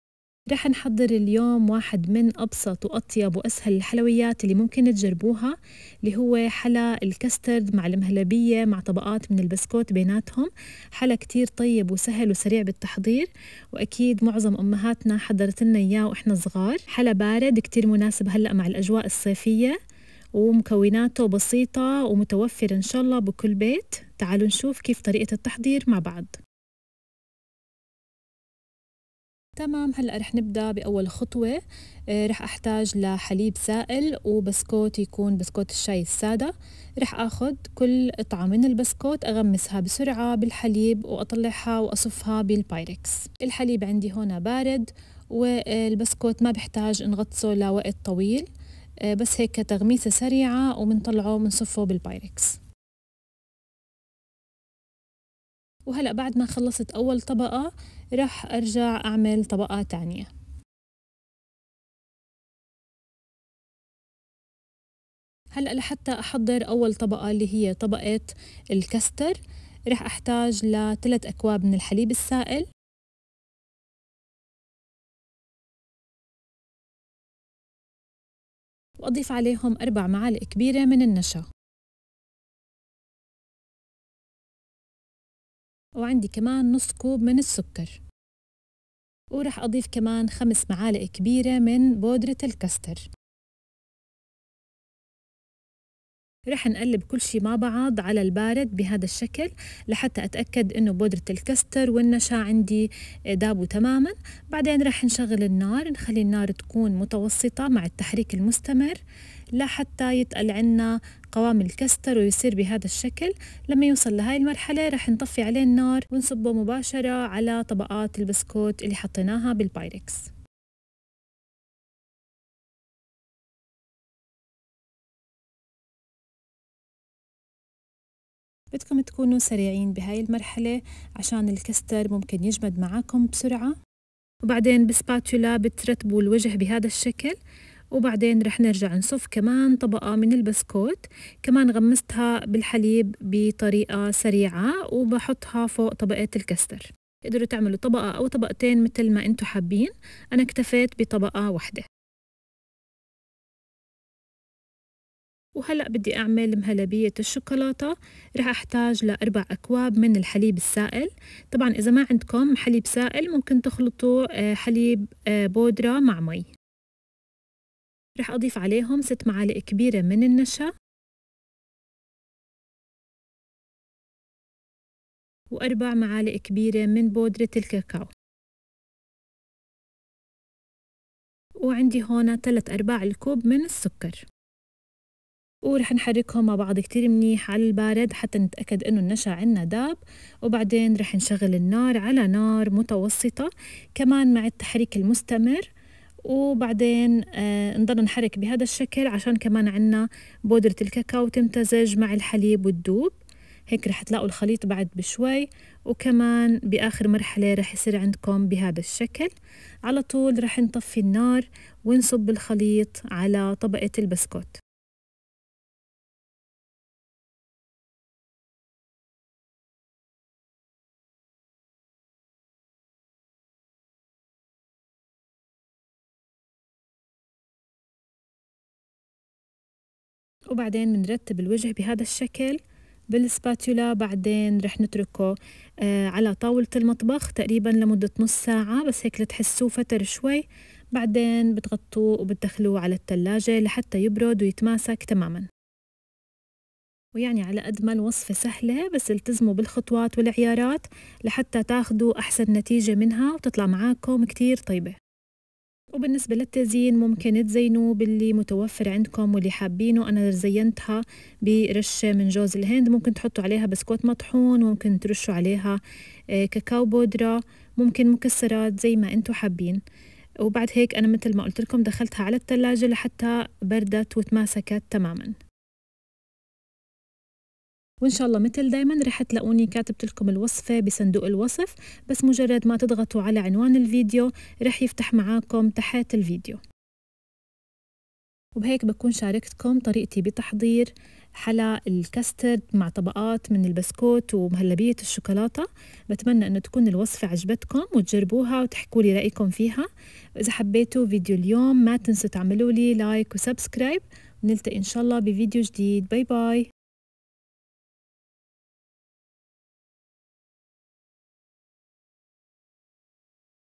حلى الكاسترد والبسكوت و مهلبية الشوكولاتة بطريقة بسيطة و بدون فرن custard layers biscuits
المكونات:
بسكوت سادة
حليب

لتحضير طبقة الكاسترد:
3 كوب حليب سائل
4 معالق كبيرة نشا
نص كوب سكر
5 معالق كبيرة بودرة الكاسترد

لتحضير طبقة مهلبية الشوكولاتة:
4 كوب حليب سائل
6 معالق كبيرة نشا
4 معالق كبيرة بودرة الكاكاو
3/4 كوب سكر

Ingredients:
Plain biscuits
milk

To prepare the custard layer:
3 cups liquid milk
4 tablespoons starch
Half a cup of sugar
5 tablespoons custard powder

To prepare the chocolate pudding layer:
4 cups liquid milk
6 tablespoons starch
4 tablespoons cocoa powder
3/4 cup sugar

حلى طبقات المهلبية و الكاسترد و البسكوت